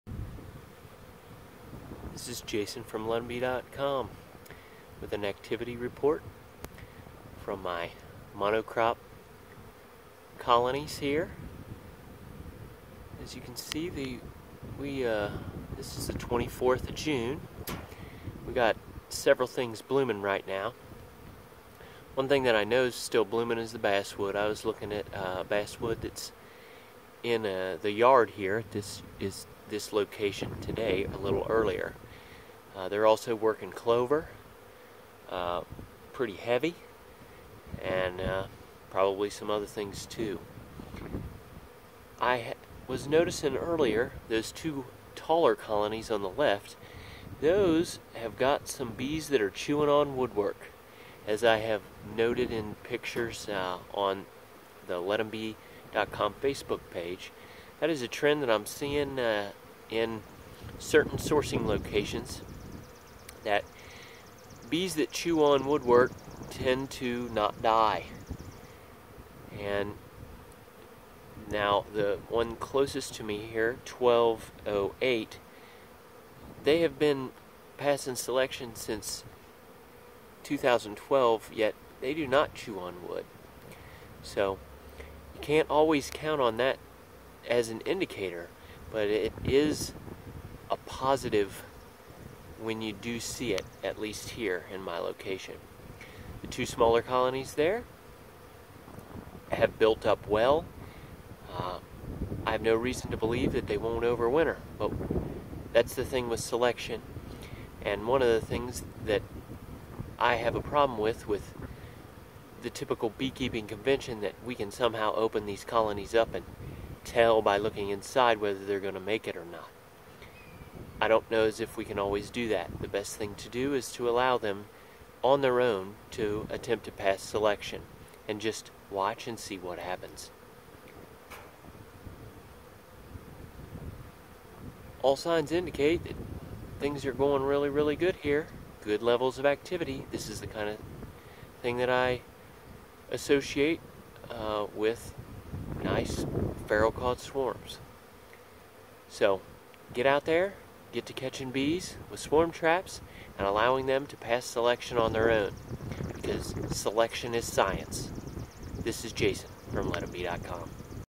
Selection is SCIENCE! Hive Activity Report 2017-06-24
Colonies from LEFT to RIGHT:  1504, 1704, 1505, 1703, 1208.  Naming convention = [YEAR colony was trapped] [# in sequence of swarm capture for that year]  1504 was the FOURTH colony captured in 2015.

Conditions were good at this outyard today.  All colonies were displaying good activity.  It is apparent that all colonies were more or less consistent in their traffic.  This is an important aspect of outside observation.  When there are several colonies at one site they can be compared.  Any colonies that appear to be lagging behind may need to be watched closely for failure.  At this point it's not a concern at this site.

It has recently been noted that 1504 and 1505 (the two taller colonies on the left side of the stand) are chewing the wood around the entry holes in the deep hive bodies.  This has been shown to be a good prognostic indicator of successful productive colonies here in Eastern Indiana.  I have spoken with other area keepers that have made similar observation.

The two 8-frame colonies, 1704 and 1703 (double deeps) that were hived this year continue to show signs of population increase.  Neither has been opened since being hived back in April.  In my yards such colonies are not opened until the following Spring unless they show signs of failure.  Their only job is passing selection this winter so in order to give them the best chance possible they are not to be messed with.

This is my first year experimenting with 8 frame equipment and thus far all signs indicate that they are taking to them well.  There's a good chance I may start 10 more of them next year.  Next year the first 10 will have their first chance at honey production which is another experiment I'm excited about.

Selection is being used to staff this hive-stand.  I do not believe I have the ability to determine with 100% accuracy which colonies will pass selection through INVASIVE hive inspections.  Given this as a KNOWN part of the experiment I am confident in allowing selection alone yield the outcome.  I will continue to follow along with this yard into the future to document the outcome of the selective process.

To learn more go to LetMBee.com or the LetMBee.com Facebook page.